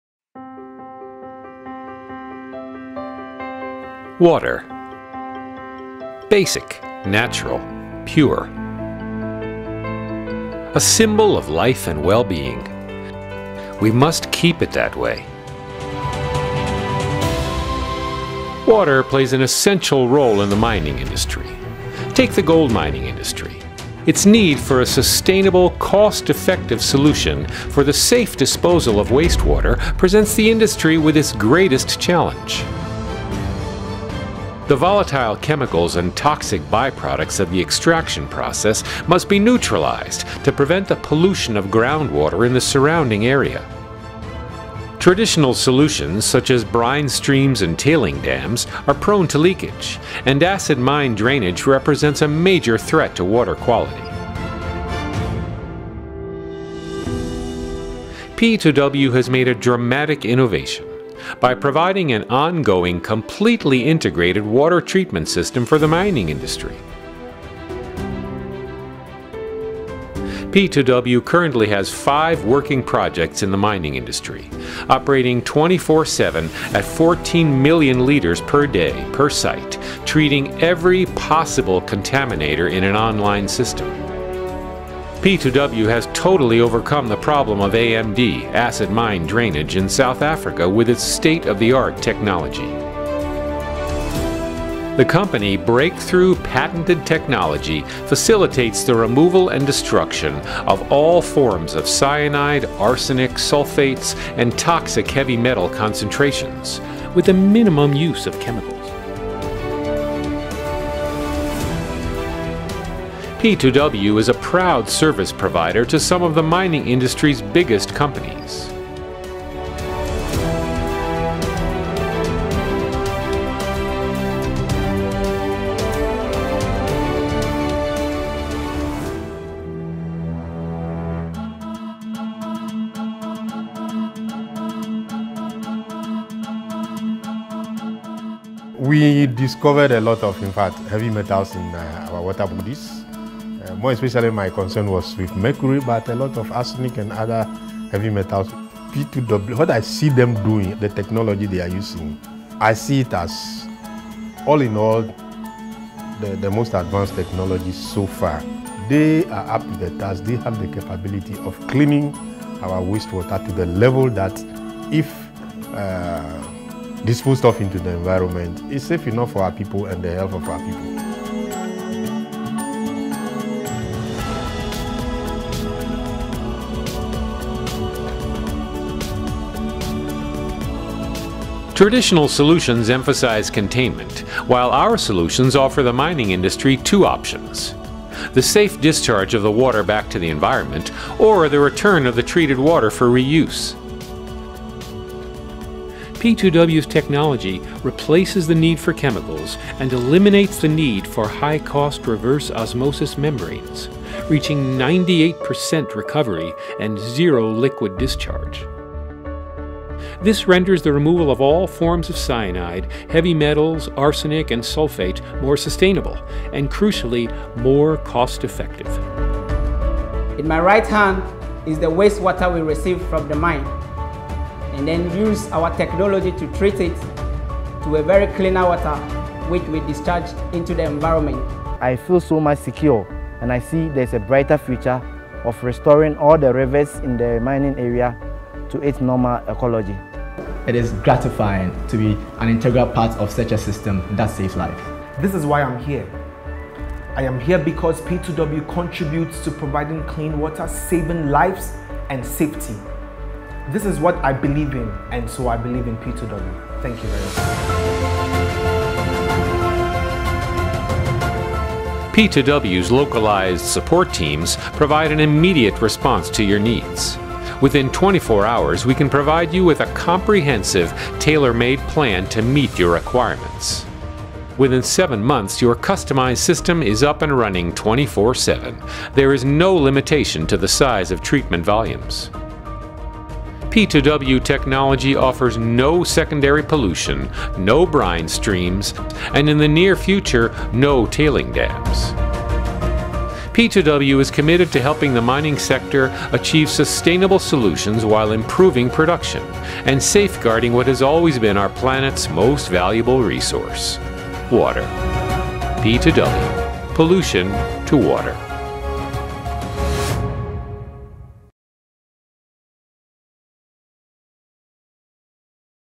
P2W" Image Film - Water and Wastewater Treatment for the Mining Industry" - 2021 - English
P2W was established in 2005. For over15 years, P2W has accomplished top-quality installations accompanied by full O&M track record in the area of Mining, PCB, Batteries, Plating, Underground Water and Municipal. P2W Operates and maintains three of the most advanced and sustainable plants for the Mining industry, working with some of the leading corporations of mining in the world. With HQ in Israel and Ghana, P2W's advanced technology for the mining industry treats more than 14,000,000 liters per day, per site, global-wide.